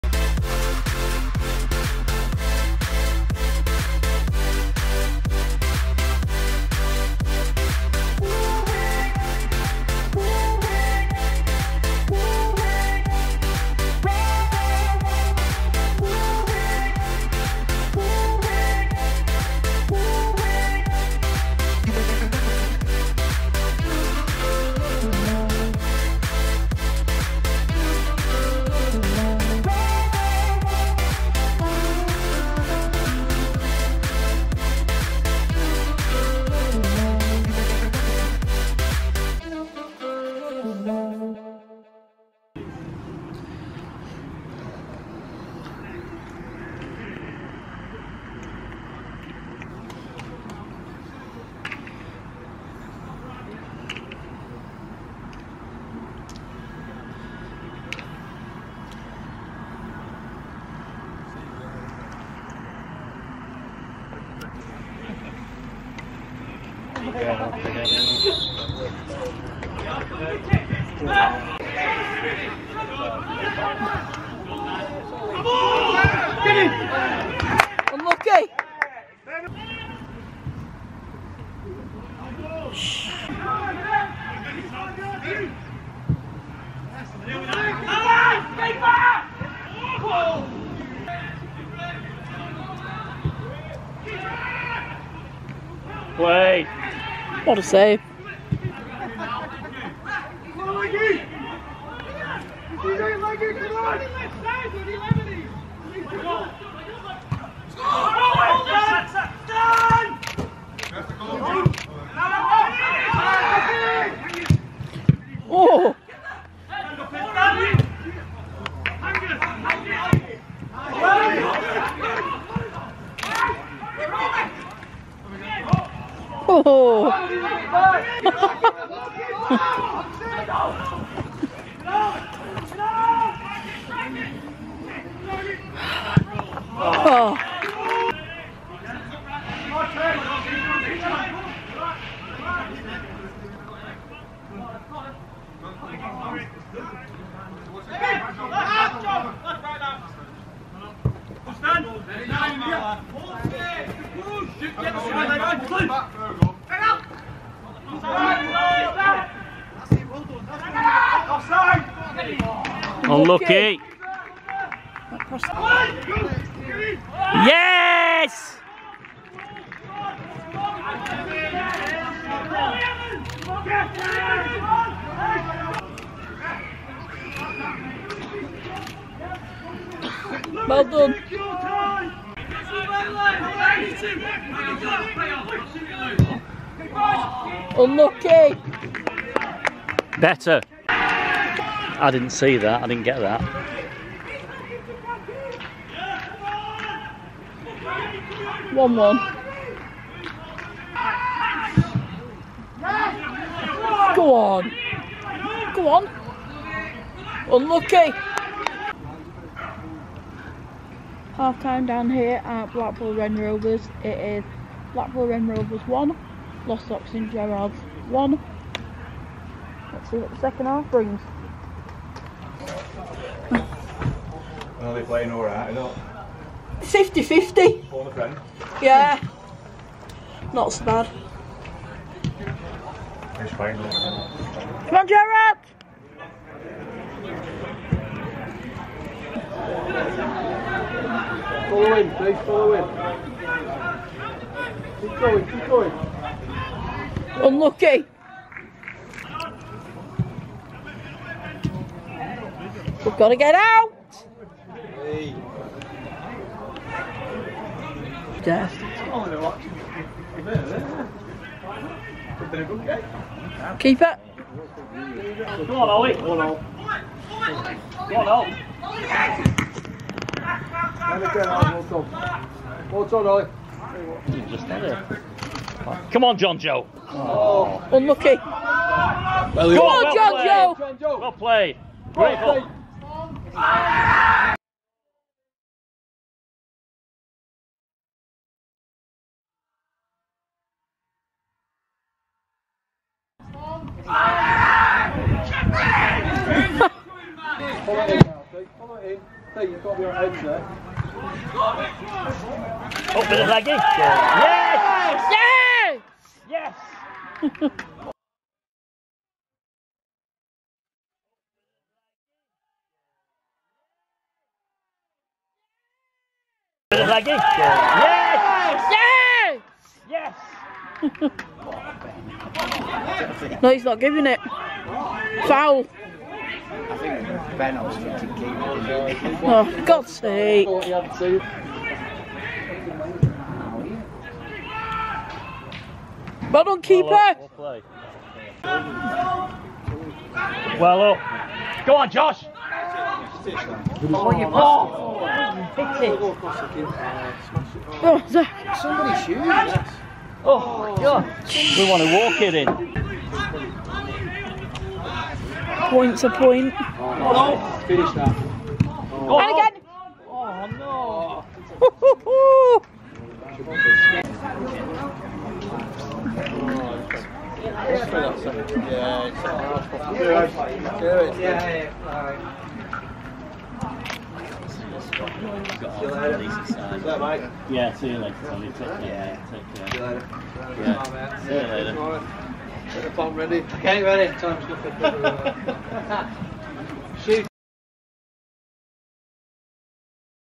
POINTS ARE SHARED! Blackpool Wren Rovers vs Gerards
Hello and welcome to a brand new Lostock St Gerards video i hope you enjoy this one x

Tuesday night we take the trip to Blackpool Wren Rovers for gerards 2nd away game can we claim another 3 points and carry on our good form!

FULL TIME
Blackpool Wren Rovers 1
Losotck St Gerards 1

Natalie